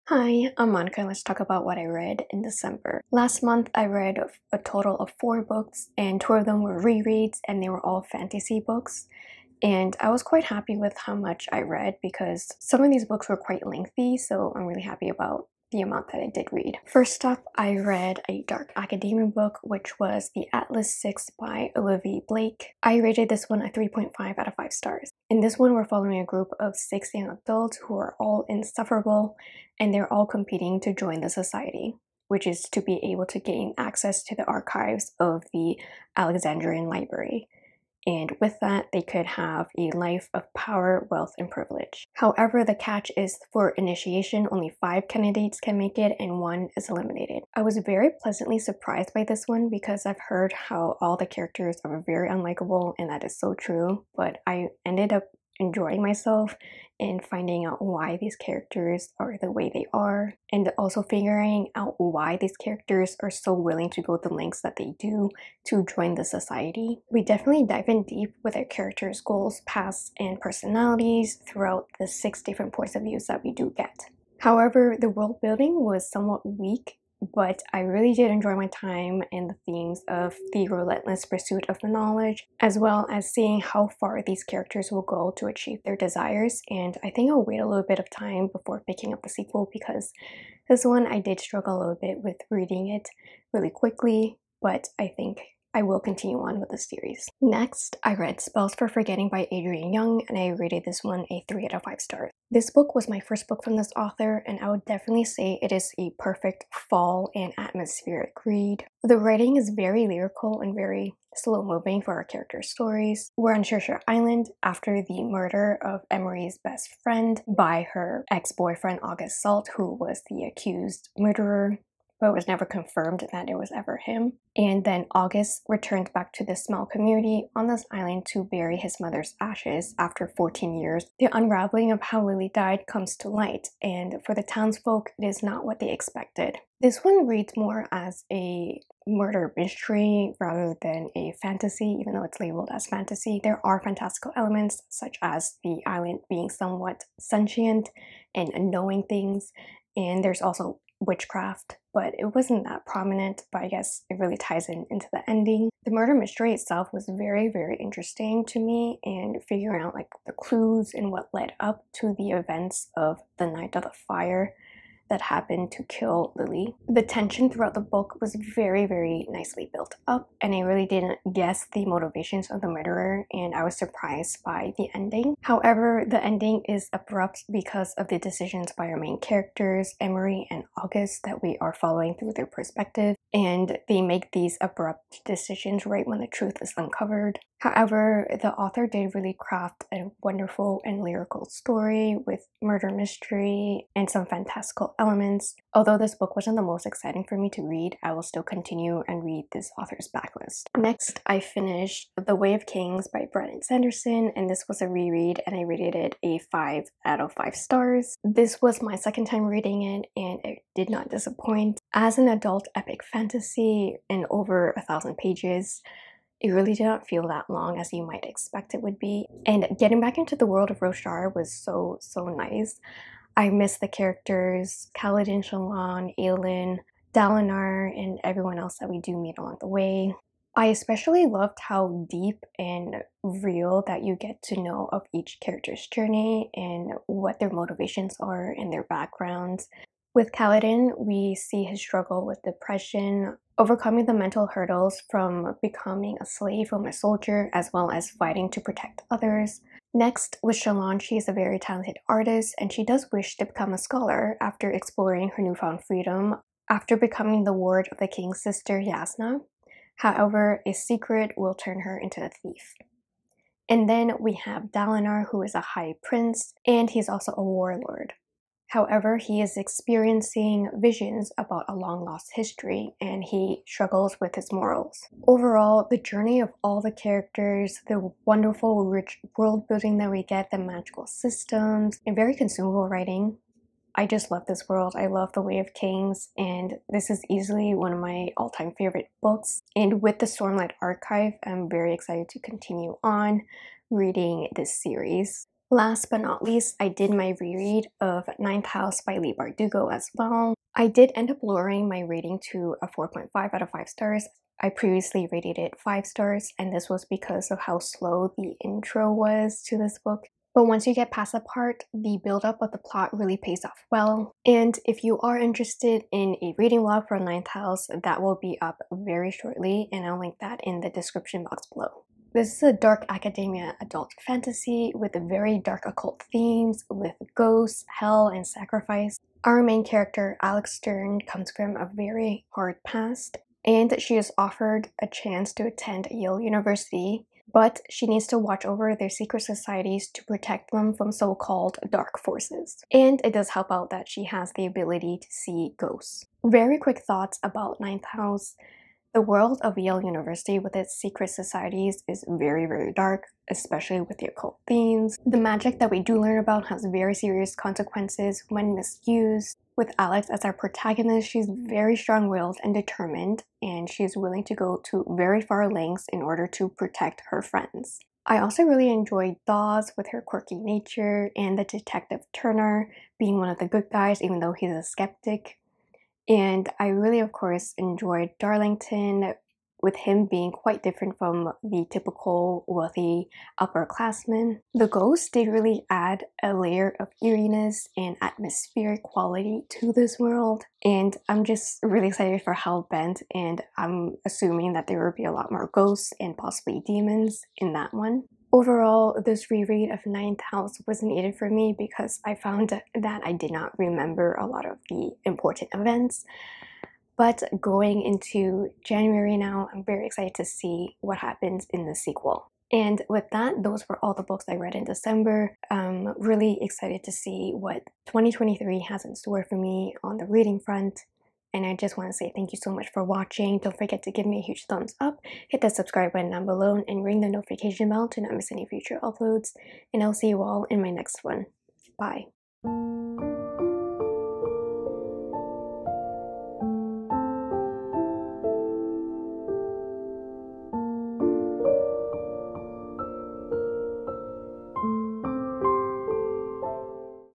the last books of 2022! | December Wrap Up 2022
Hello friends! I'm sharing the books I read in December, enjoy!

Timestamps:
0:00 Intro
0:19 The Atlas Six
1:37 Spells for Forgetting
3:22 The Way of Kings
4:55 Ninth House
6:52 Outro
Books Mentioned:
The Atlas Six by Olivie Blake
The Way of Kings by Brandon Sanderson
Ninth House by Leigh Bardugo
Spells for Forgetting by Adrienne Young